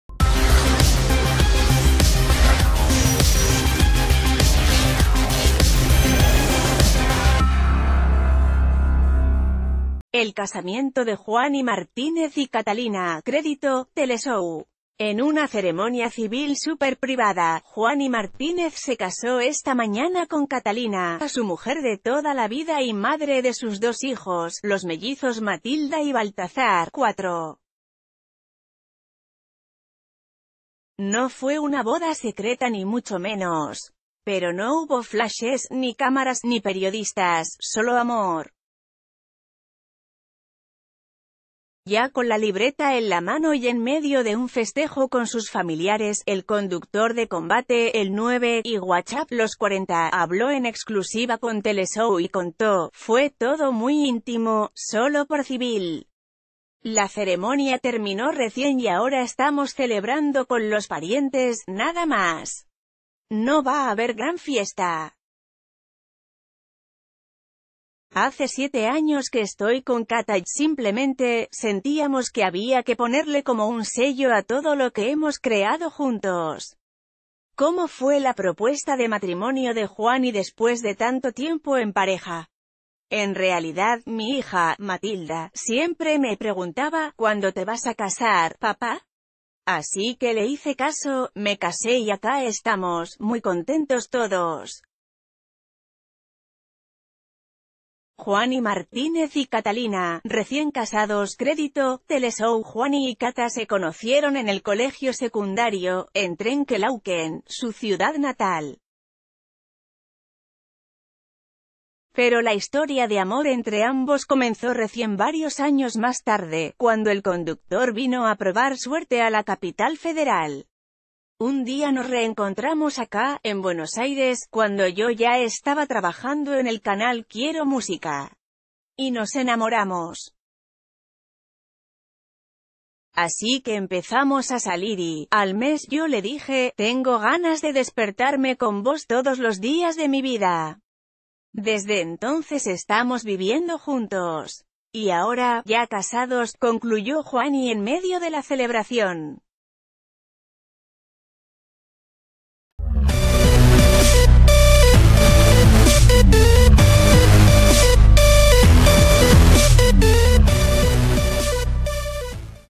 Se casó Juani Martínez con su novia de toda la vida: "Queríamos ponerle un sello a todo lo que he...
Se casó Juani Martínez con su novia de toda la vida: "Queríamos ponerle un sello a todo lo que hemos creado juntos"

El casamiento de Juani Martínez y Catalina (Crédito: Teleshow) En una ceremonia civil súper privada, Juani Martínez se casó esta mañana con Catalina, su mujer de toda la vida y madre de sus dos hijos, los mellizos Matilda y Baltazar (4). No fue una boda secreta ni mucho menos. Pero no hubo flashes, ...